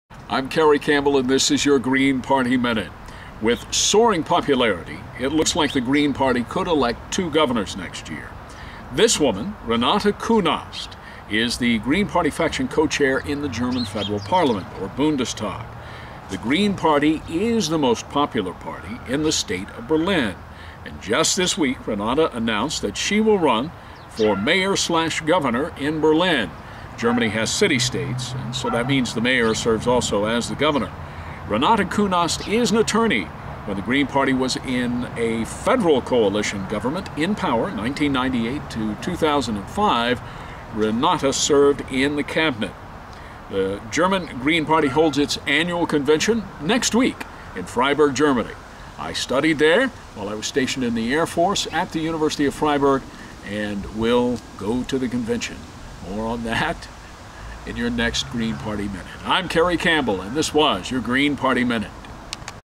Carey Campbell's Green Party Minute
Carey Campbell reports on Green Party popularity and chance to elect 2 Governors in 2011.